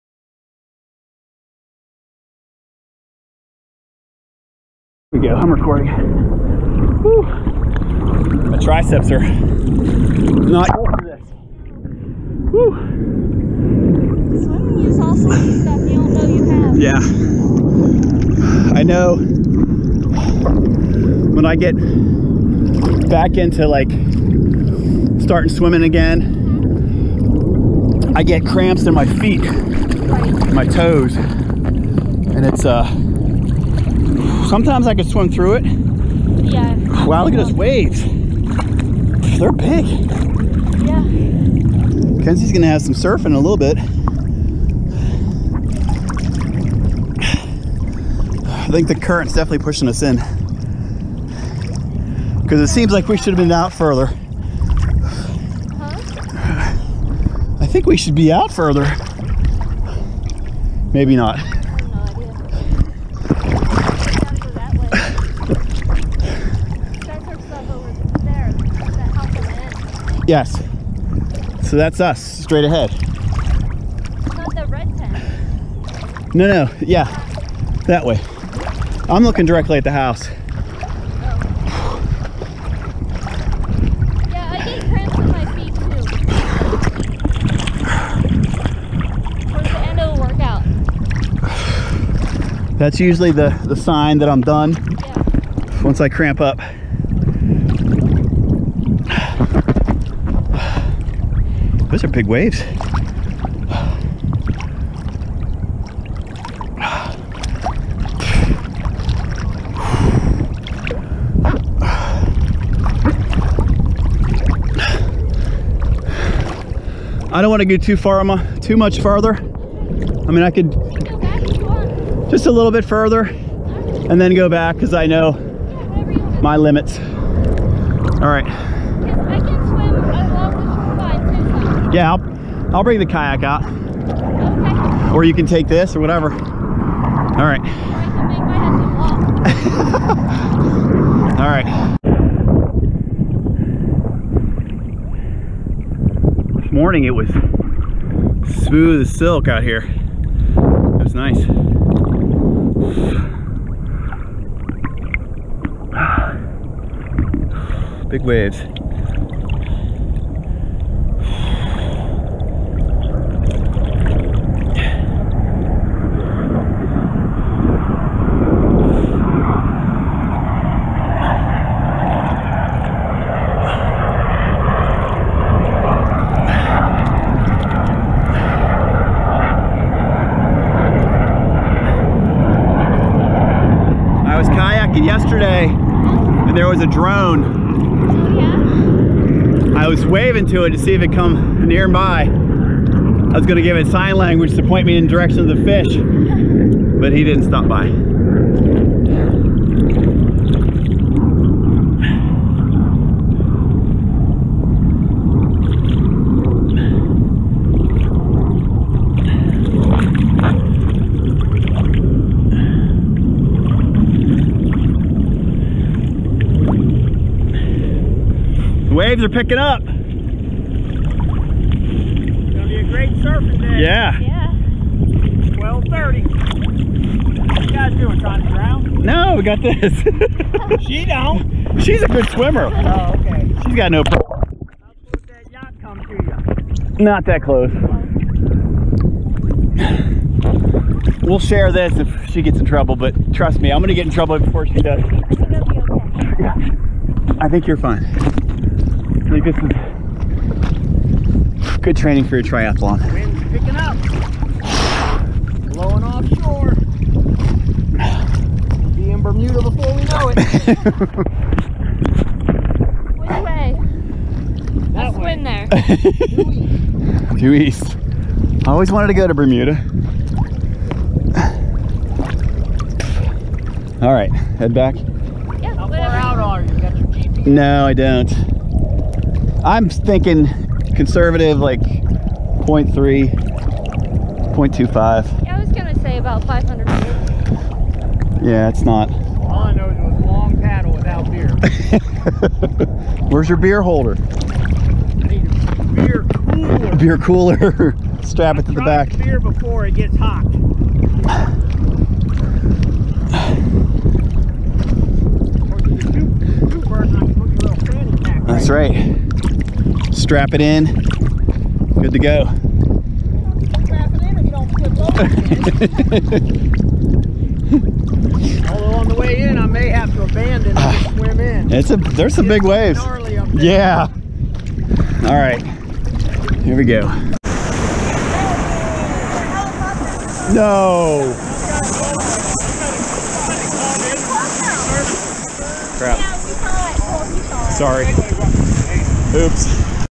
Open Water Swim Carova, NC 2018
I was testing out my new swim buoy with our friend Susan.